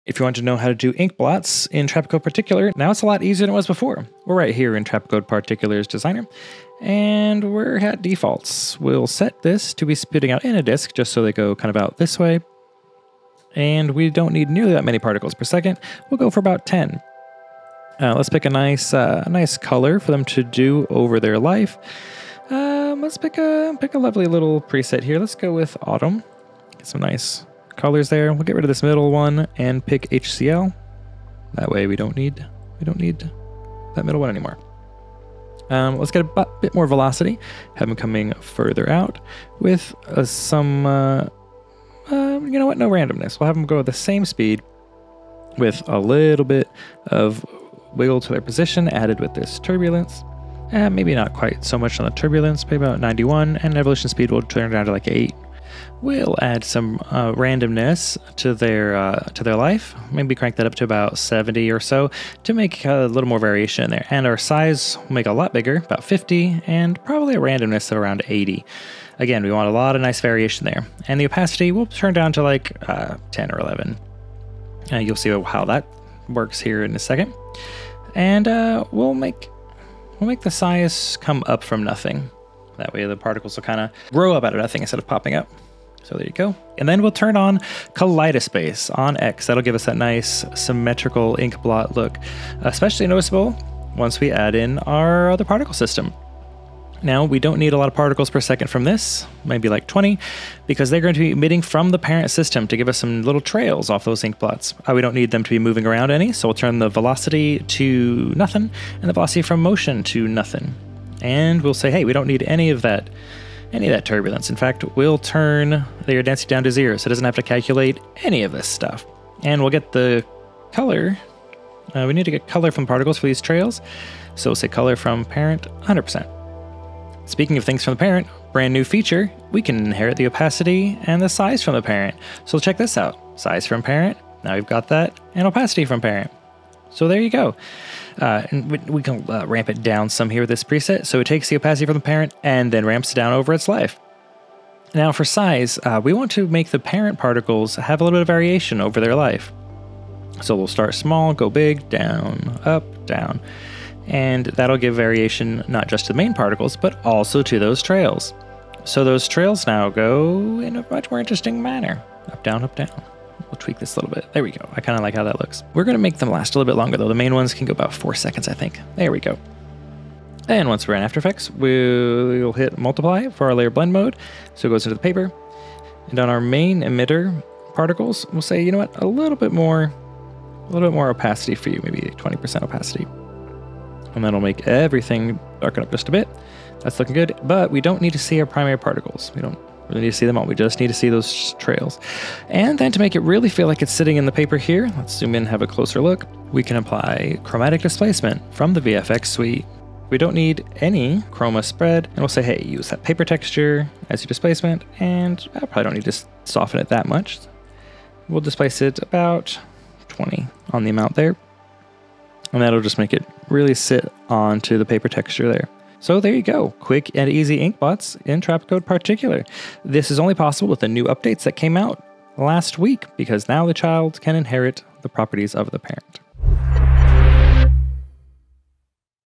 Ink Blots in Trapcode Particular Tutorial
Want to make ink blots, splotches, and bleeds? Perhaps your own Rorschach test? @thegreatszalam  (Michael Szalapski) takes you through some of the new features in Trapcode Particular to help you get them, fast! Now that child particles can inherit the size and opacity of the parent particles, it's easy to create more organic looks. Useful for motion graphics and visual effects!
Also, a quick tip on how to use Chromatic Displacement to help your ink sit into the paper texture.